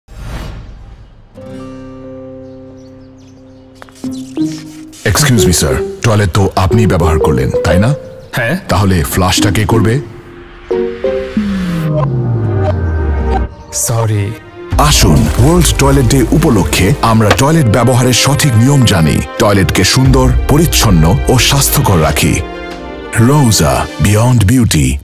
Think Before Leaving
টয়লেট আমাদের সবার কাছেই অত্যন্ত গুরুত্বপূর্ণ ও ব্যক্তিগত স্থান। এখানে নোংরা, অস্বাস্থ্যকর পরিবেশ কখনই কাম্য নয়। তাই এবারের ওয়ার্ল্ড টয়লেট ডে উপলক্ষ্যে, ব্যবহারের সঠিক নিয়ম জেনে টয়লেটকে শুধু সুন্দরই নয়, সাথে পরিচ্ছন্ন ও স্বাস্থ্যকর রাখার আহবান জানাচ্ছে, রোসা।
আসুন, এখন থেকে আমরা টয়লেট ব্যবহারের পর আরেকবার ভেবে দেখি, পরবর্তী ব্যবহারকারীর জন্য জায়গাটি সুন্দর ও পরিচ্ছন্ন করে রেখে যাচ্ছি কিনা।